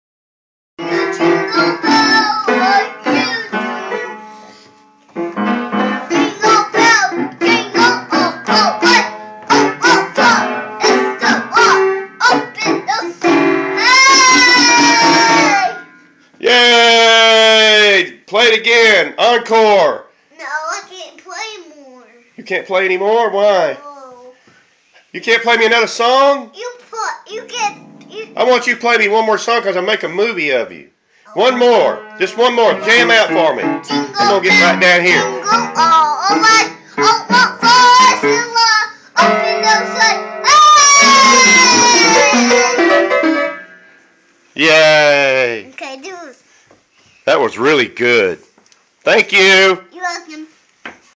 Josh Playing the Piano
Josh playing the Piano on 2/15/2013 at Rockingham Road on my old Piano built in 1966